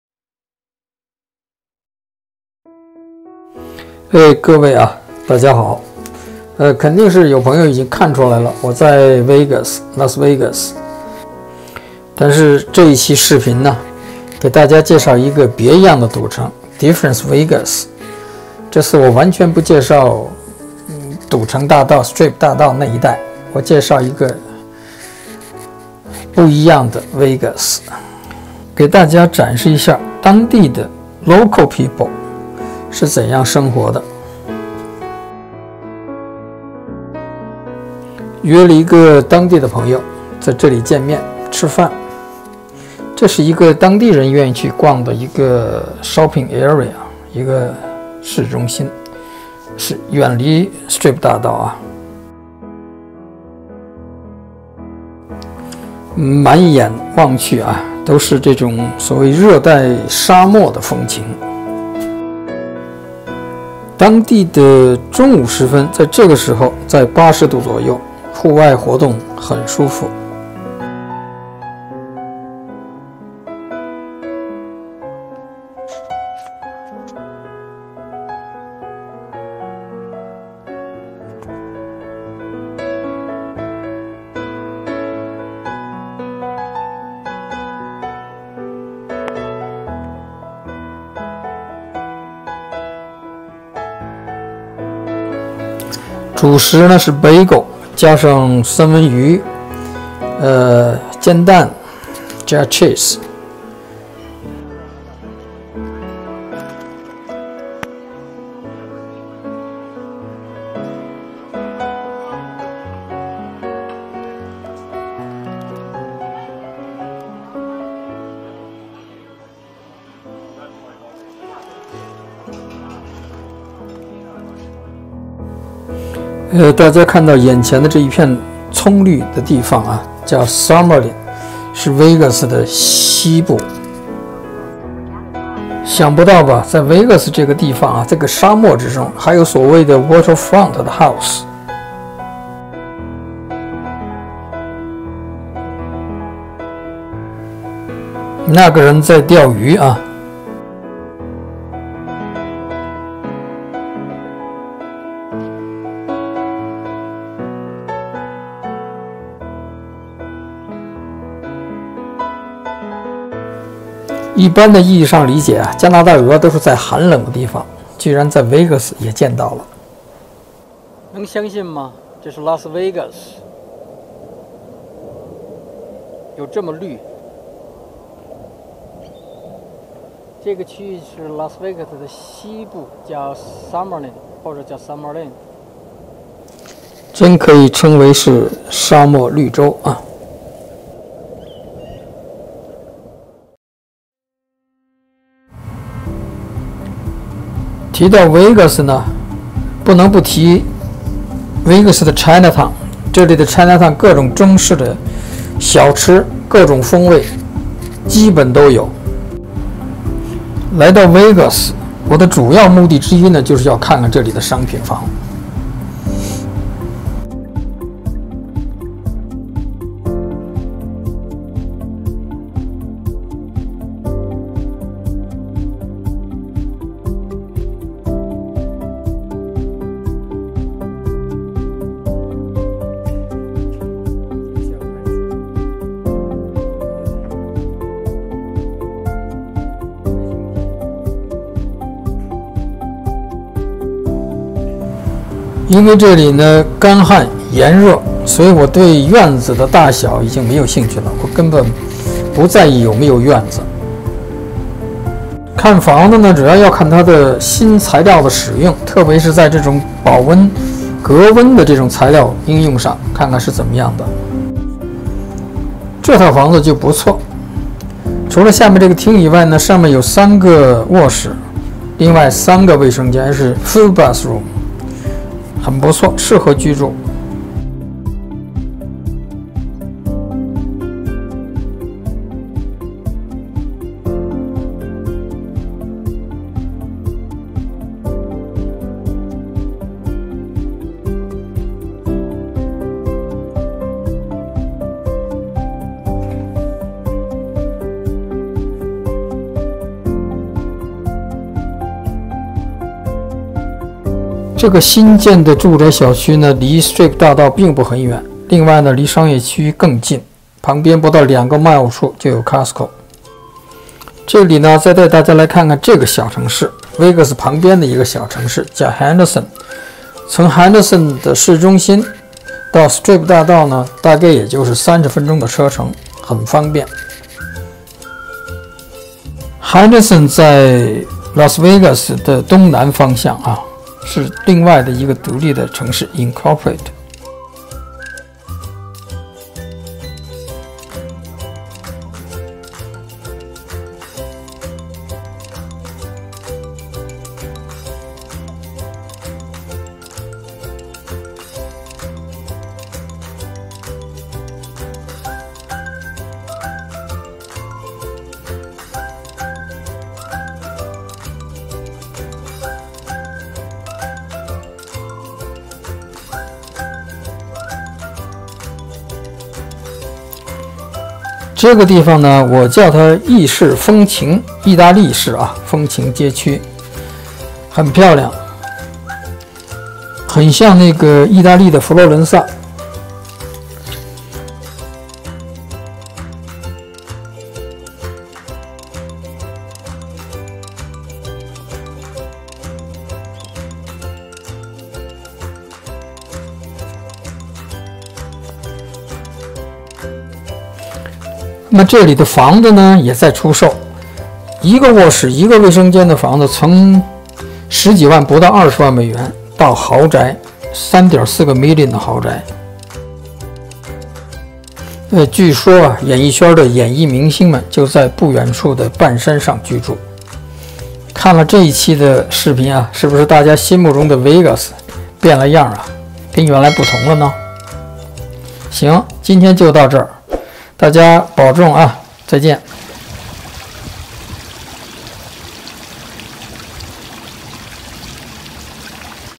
别样的赌城
美国赌城的另一张面孔，当地人的居住与生活，沙漠中的绿洲